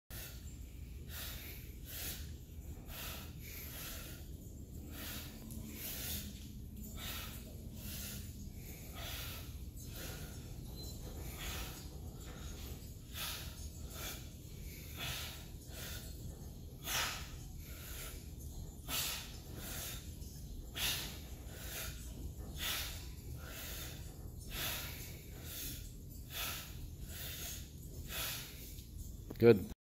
Seated 360's with a short mace
This exercise can be done standing or seated.
We did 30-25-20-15 reps tonight.